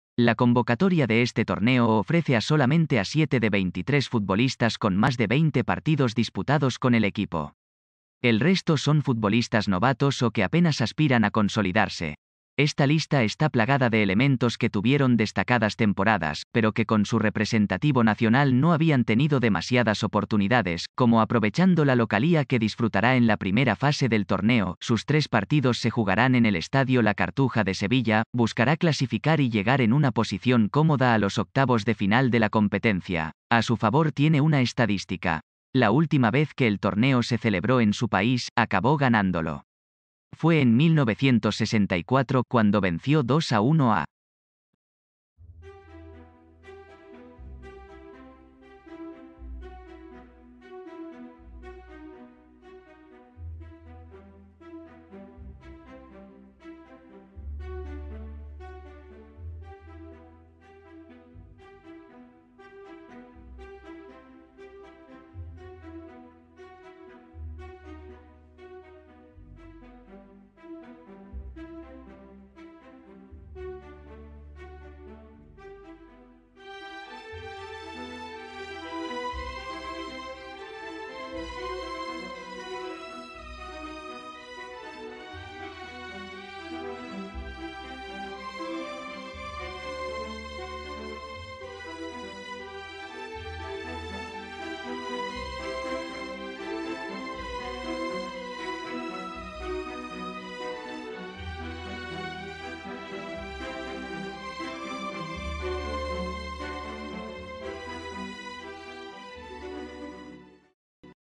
Euro 2020: España afrontará el torneo en medio de un proceso de renovación
La convocatoria de este torneo ofrece a solamente a siete de 23 futbolistas con más de 20 partidos disputados con el equipo. El resto son futbolistas novatos o que apenas aspiran a consolidarse.Esta lista está plagada de elementos que tuvieron destacadas temporadas, pero que con su [...]